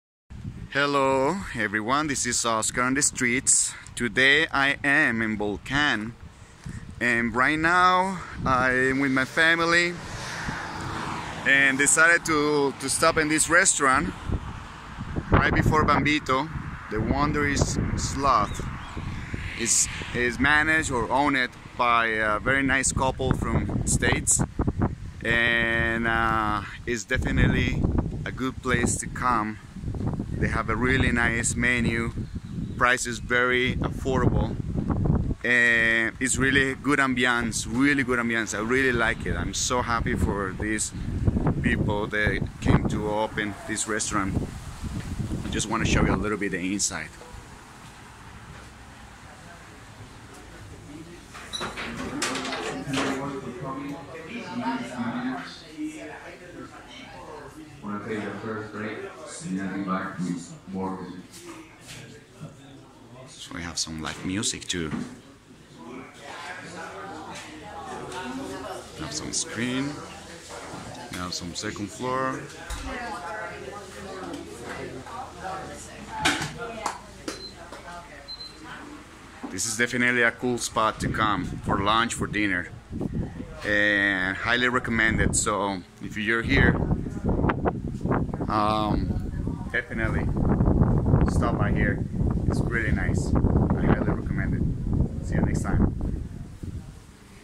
The Wandering Sloth Volcan Restaurant
Oscar visits The Wandering Sloth Restaurant in Vocan Panama